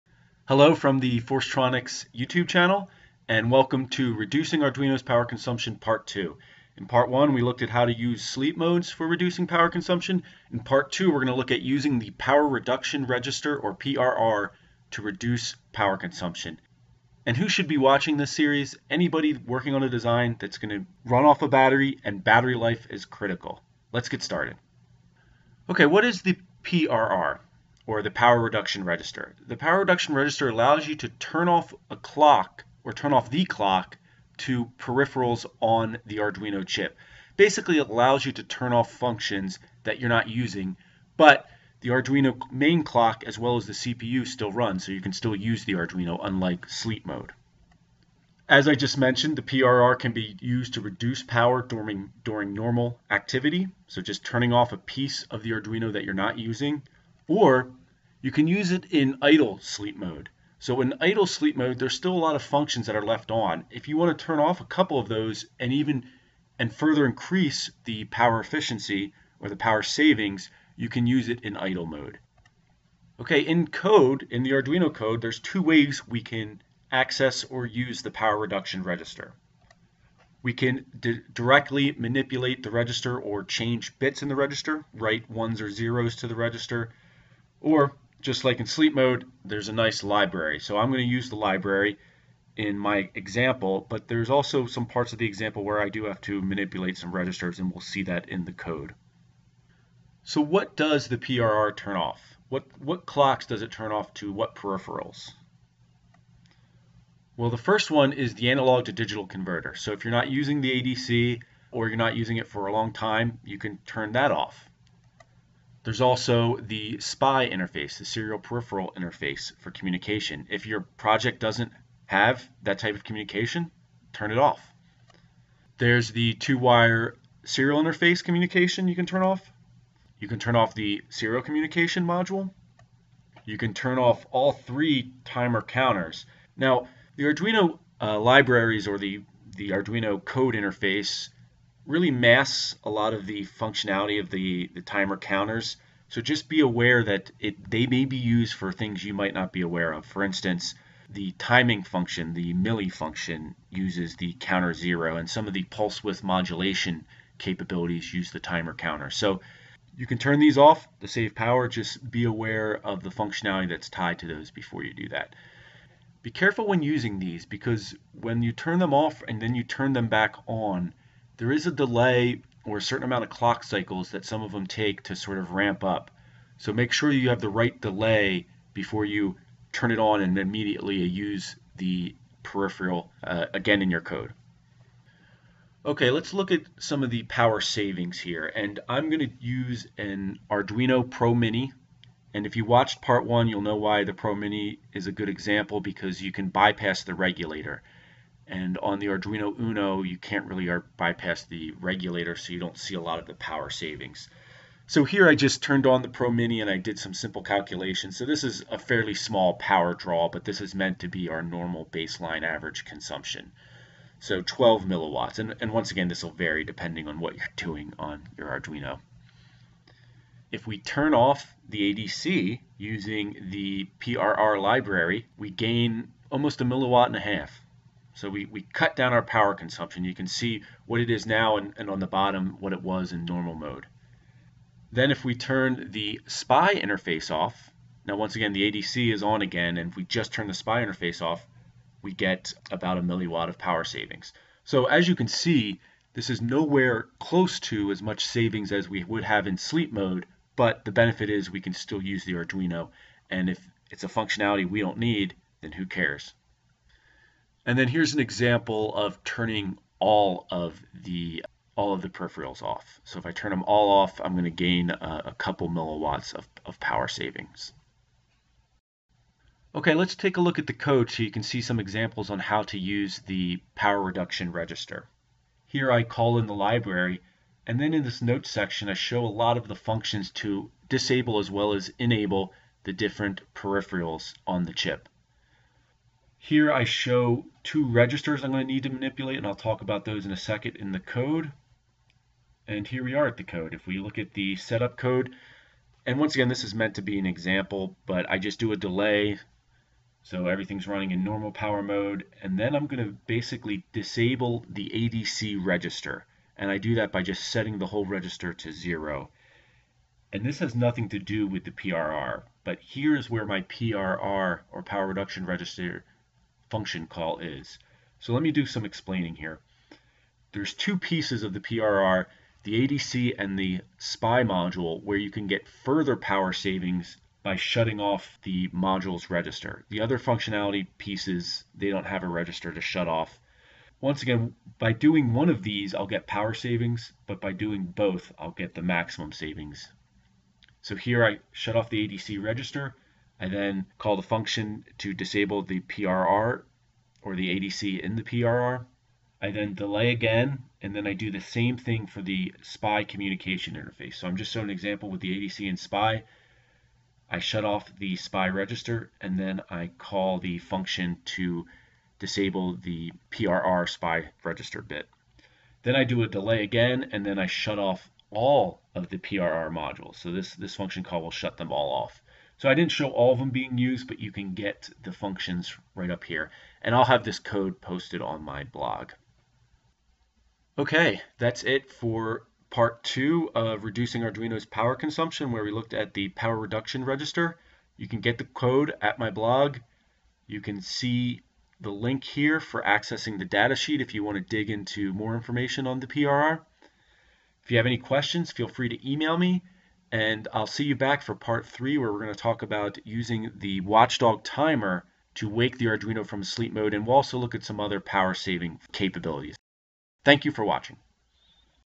Reducing Arduino’s Power Consumption Part 2
In Part two, of this three part series, we will look at how to use the Power Reduction Register (PRR) to reduce power consumption by turning off parts of Arduino that are not being used. This series is for anybody using Arduino for a battery powered project where maximizing battery life is critical. To access the code from the video go to the ForceTronics blog